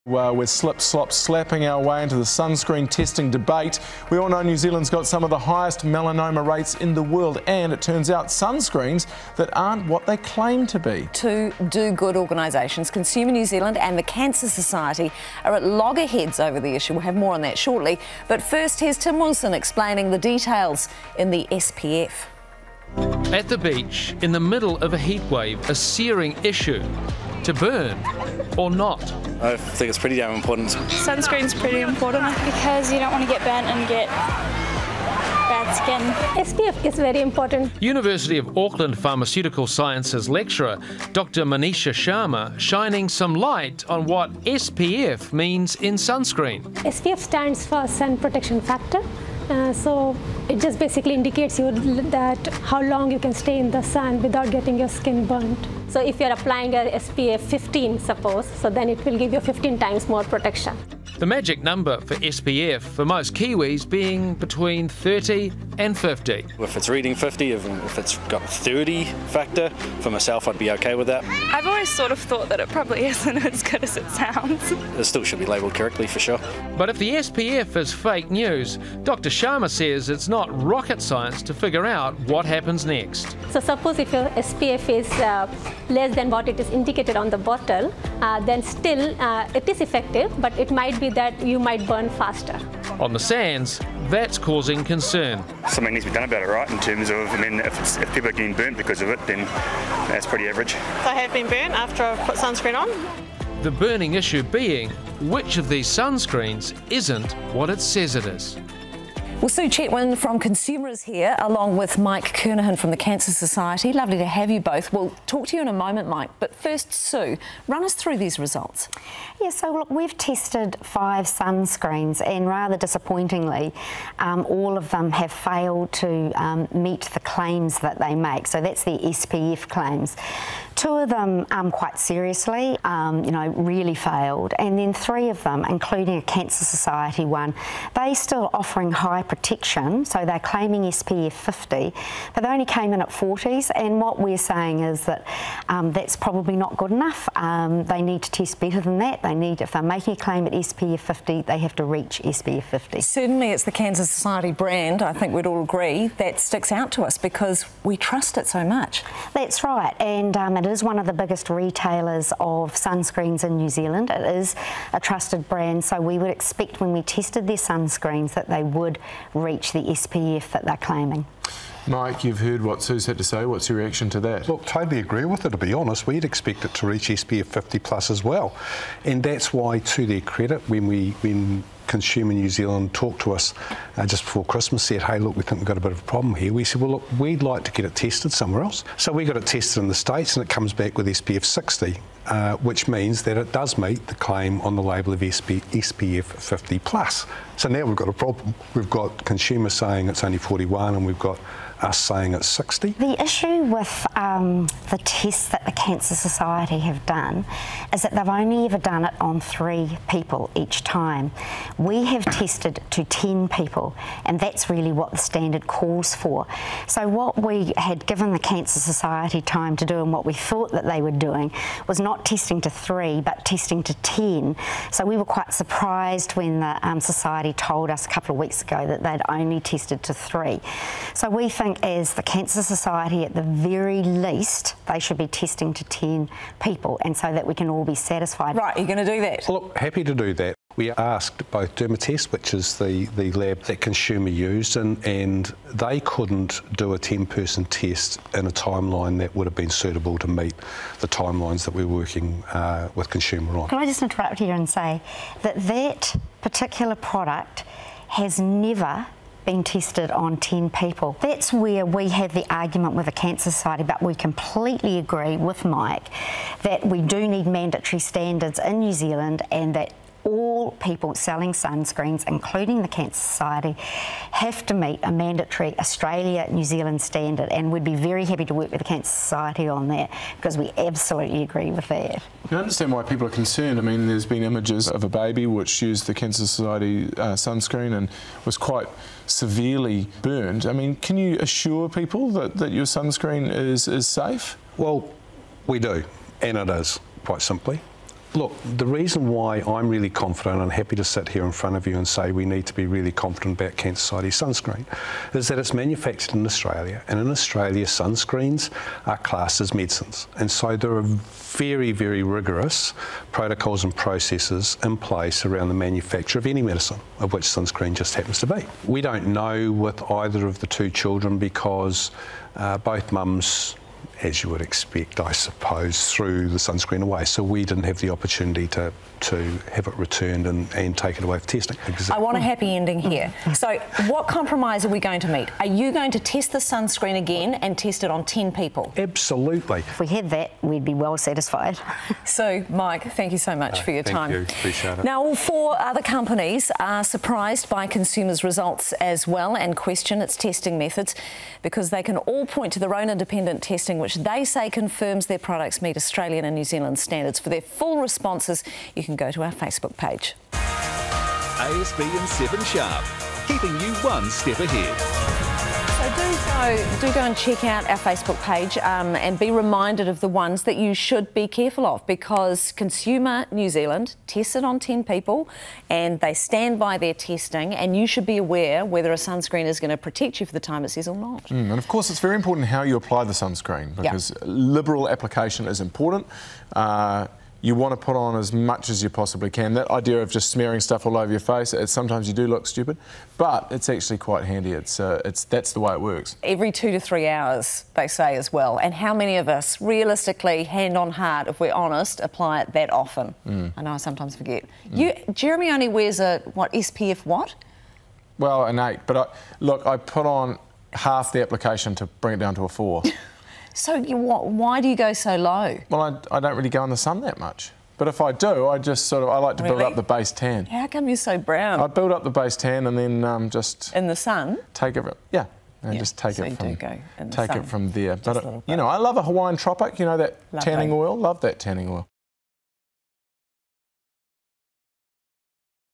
Consumer NZ reveals five sunscreens they say aren’t up to scratch
They’re at loggerheads with the Cancer Society over the issue.